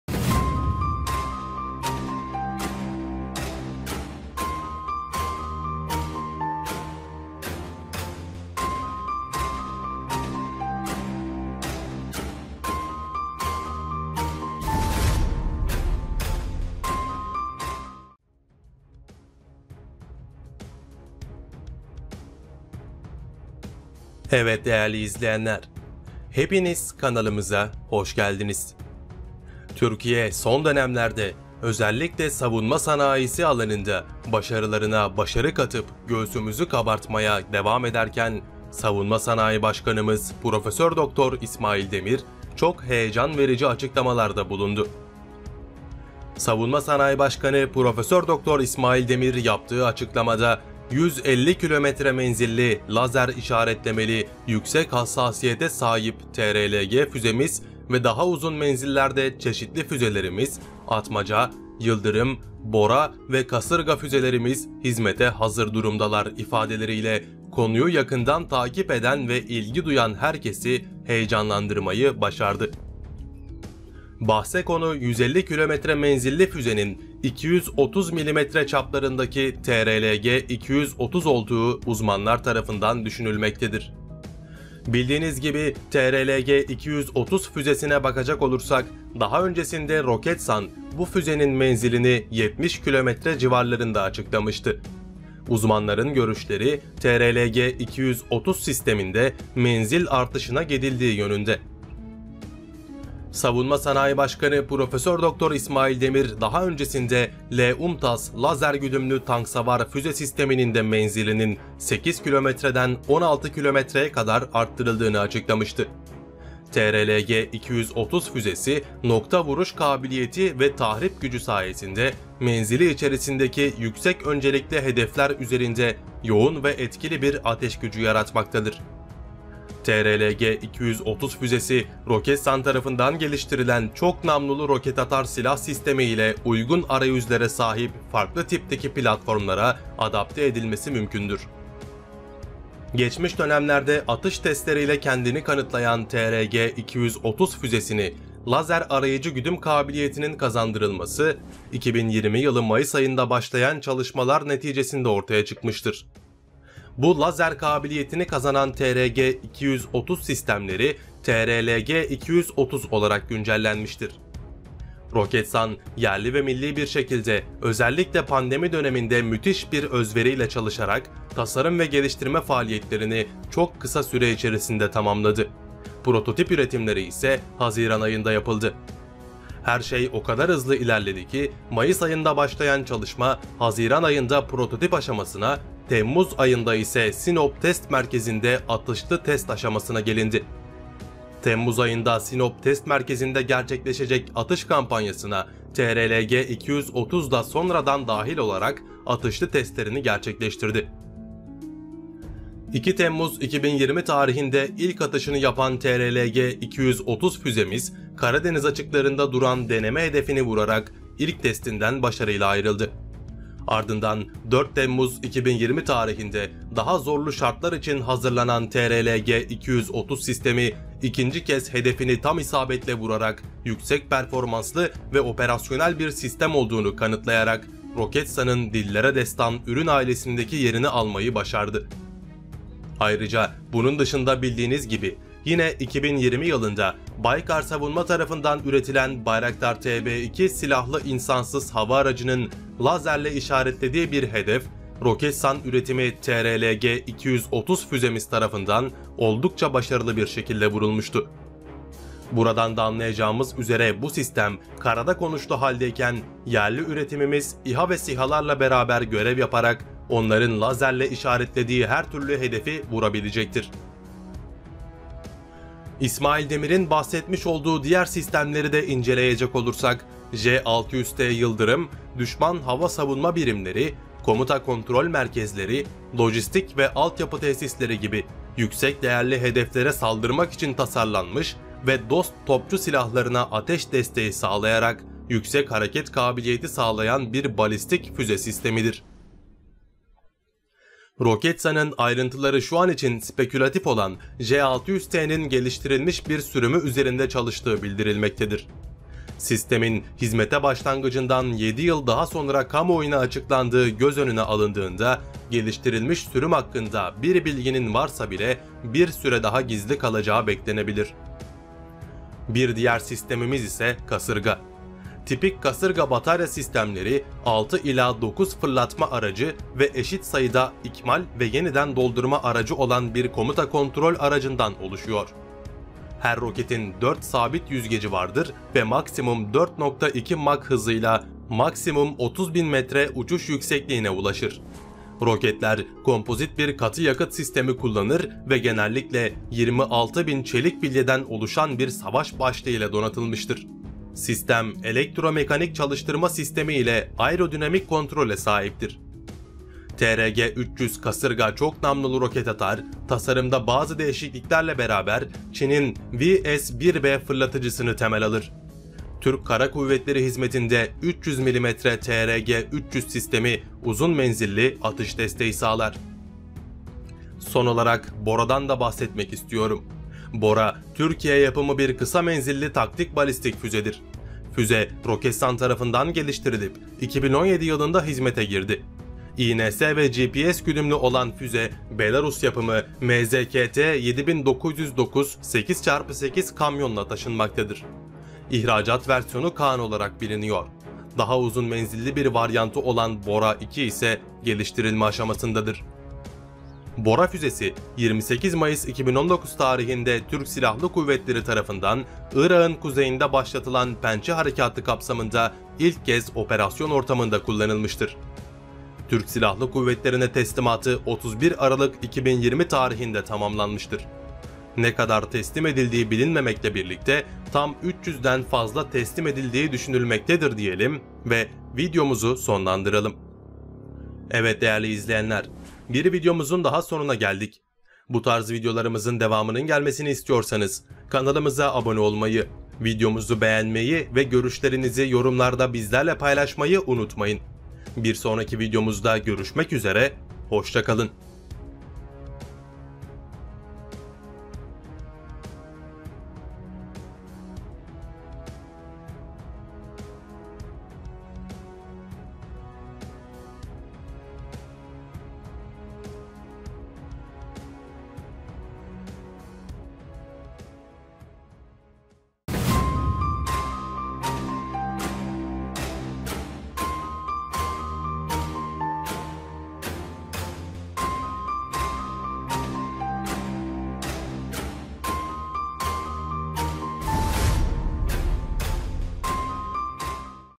UZUN MENZİLLİ DEVASA FÜZEMİZ TSK ENVANTERİNE GİRDİ ! TÜRKİYE SONUNDA BUNU DA BAŞARDI !
Türkiye son dönemlerde özellikle savunma sanayi alanında başarılarına başarı katıp göğüsümüzü kabartmaya devam ederken Savunma Sanayi Başkanımız Prof. Dr. Sayın İsmail Demir çok heyecan verici açıklamalarda bulundu. Prof. Dr. İsmail Demir yaptığı açıklamasında "150 km menzilli lazer işaretlemeli yüksek hassasiyete sahip TRLG füzemiz ve daha uzun menzillerde çeşitli füzelerimiz; ATMACA, YILDIRIM, BORA ve KASIRGA füzelerimiz hizmete hazır durumdalar" ifadeleriyle konuyu yakından takip eden ve ilgi duyan herkesi heyecanlandırmayı başardı.

Bahse konu 150km menzilli füzenin 230mm çaplarındaki TRLG-230 olduğu uzmanlarca düşünülmektedir. Bildiğiniz gibi TRLG-230 füzesine bakacak olursak daha öncesinde Roketsan bu füzenin menzilini 70km civarlarında açıklamıştı. Uzmanların görüşleri TRLG-230 sisteminde menzil arttırımına gidildiği yönünde.

Savunma Sanayi Başkanı Prof. Dr. İsmail Demir daha öncesinde de L-UMTAS lazer güdümlü tanksavar füze sisteminin de menzilinin 8km'den 16km'ye kadar arttırıldığını açıklamıştı. TRLG-230 Füzesi nokta vuruş kabiliyeti ve tahrip gücü sayesinde menzili içerisindeki yüksek öncelikli hedeﬂer üzerinde yoğun ve etkili ateş gücü yaratmaktadır. TRLG-230 Füzesi; ROKETSAN tarafından geliştirilen Çok Namlulu Roket Atar Silah Sistemi ile uygun arayüzlere sahip farklı tipteki platformlara adapte edilmesi mümkündür.

Geçmiş dönemlerde atış testleriyle kendini kanıtlayan TRG-230 füzesine lazer arayıcı güdüm kabiliyetinin kazandırılması 2020 yılı Mayıs ayında başlayan çalışmalaran neticesinde ortaya çıkmıştır. Bu lazer kabiliyetini kazanan TRG 230 sistemleri TRLG-230 olarak güncellenmiştir. Roketsan yerli ve milli bir şekilde özellikle pandemi döneminde müthiş bir özveriyle çalışarak tasarım ve geliştirme faaliyetlerini çok kısa süreler içerisinde tamamladı. Prototip üretimleri ise Haziran ayında yapıldı.